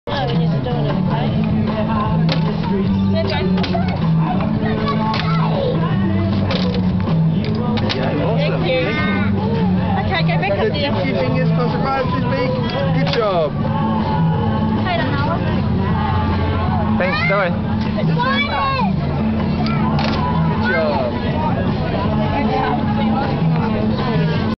Birds Show Taronga Zoo
Kael and Tanisha give coins to a bird